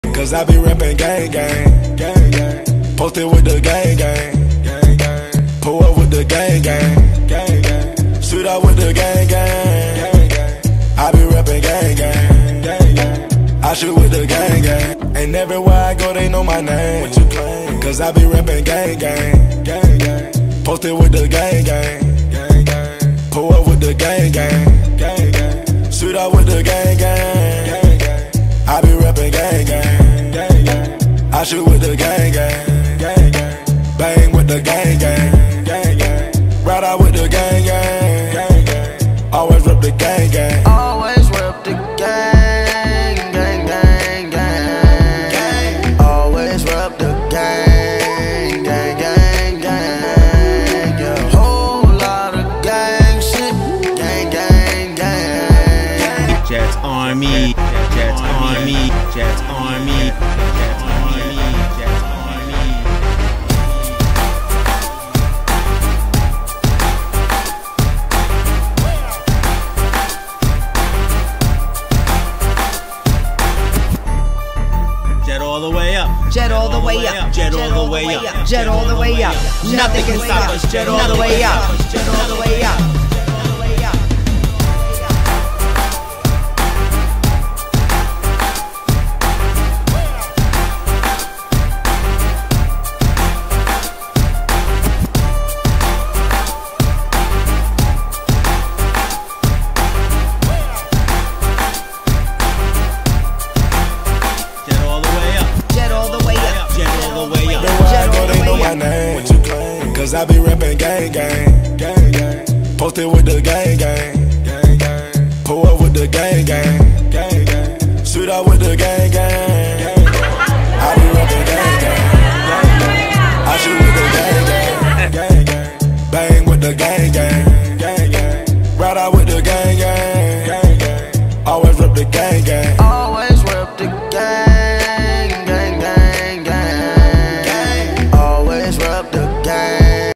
Happy Birthday Captain Jet! JETS ARMY @ Brixx & Barley Long Beach NY!
Facebook.com/JETSARMY
New York Jets Fans Club -
Tailgates, Game Watch Parties,
JETS ARMY TV, Charity events, Player Meet & Greets.